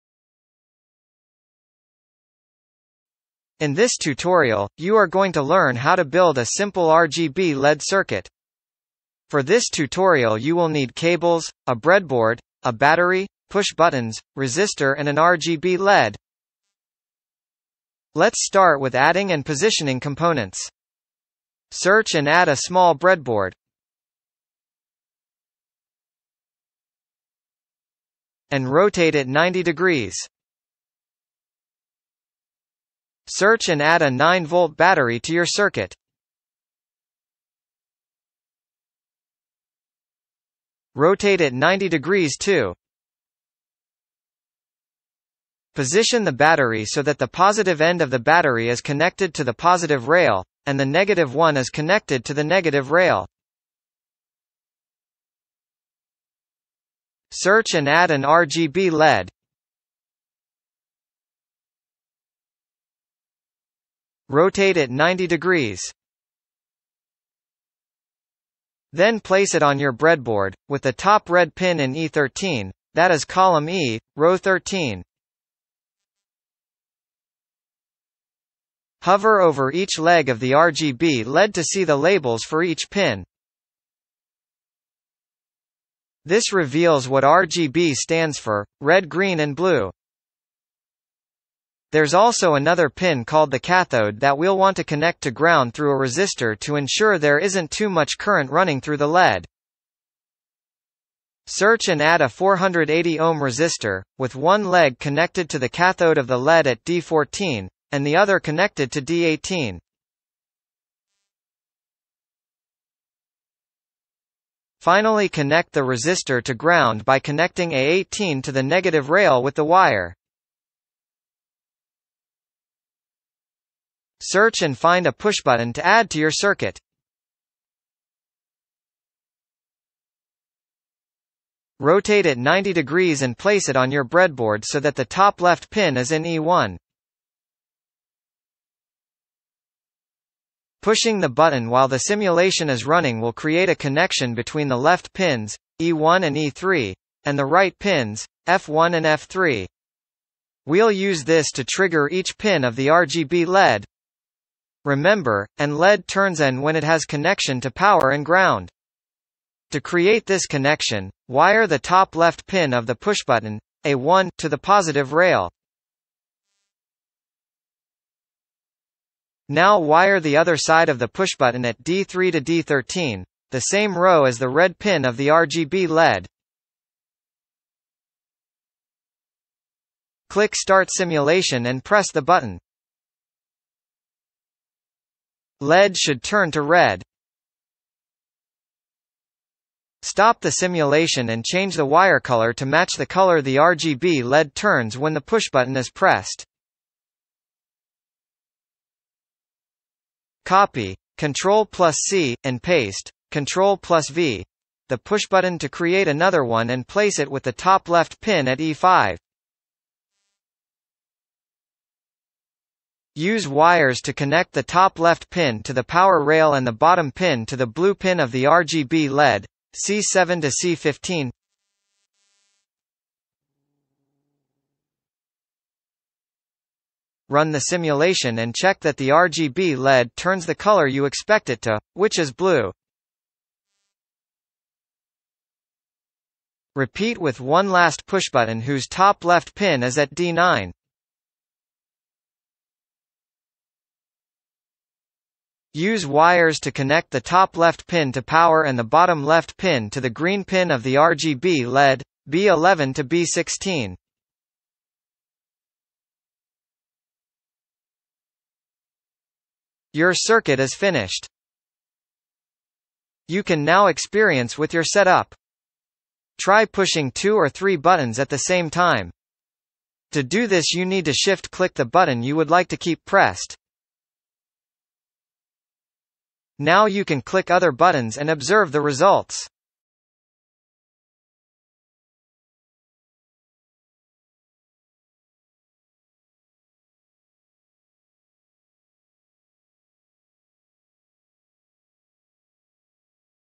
TINKERCAD | RGB LED CIRCUIT with PUSHBUTTONS
In this tutorial, you’ll learn how to design an RGB circuit with pushbutton control in TINKERCAD.

You will need:
- a breadboard
- an RGB led
- a 9V battery
- resistor
- pushbuttons